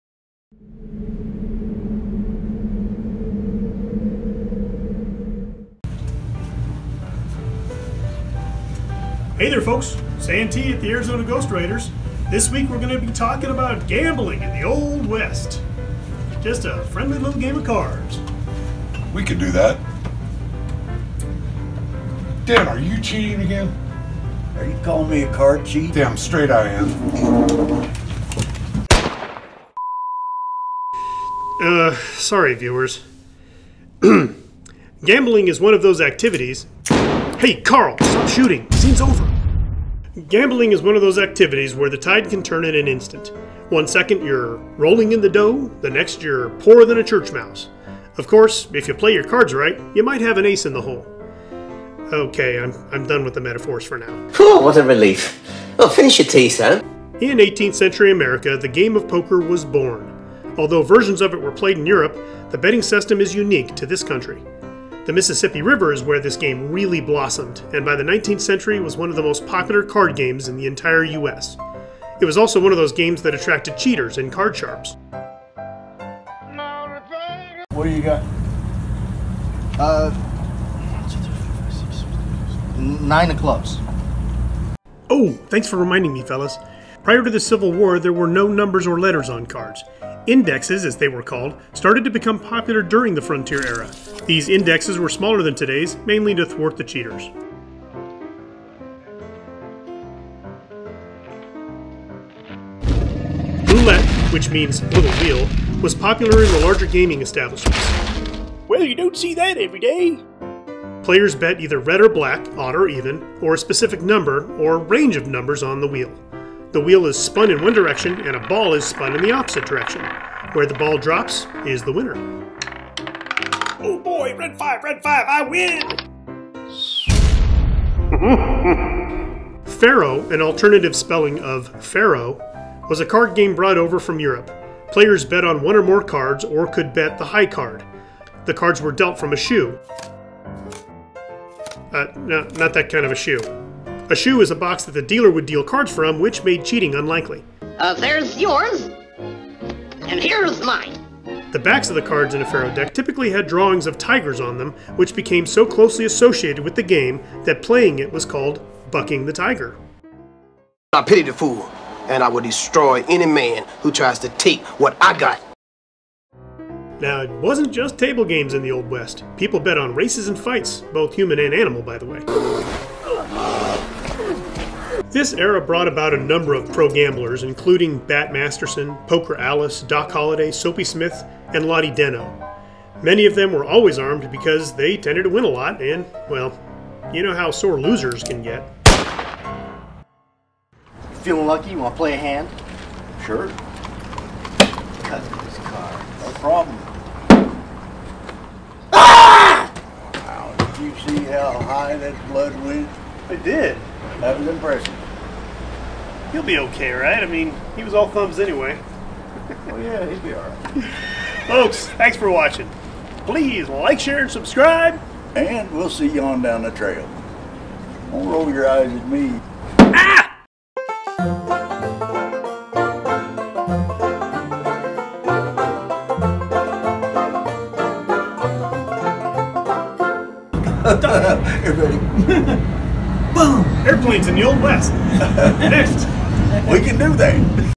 Gambling in the Old West
An overview of gambling in the Old West.